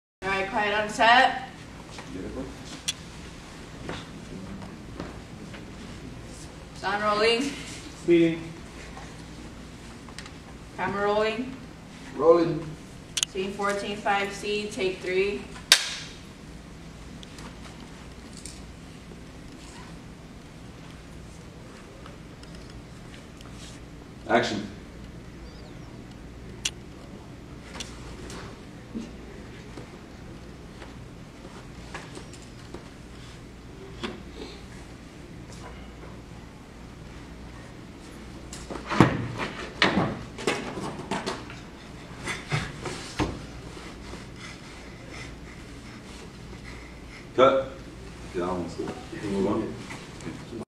Bassett High School film students
Take a behind-the-scenes look at Bassett High School's film students producing their film, to be showcased at the Creative Artists Agency!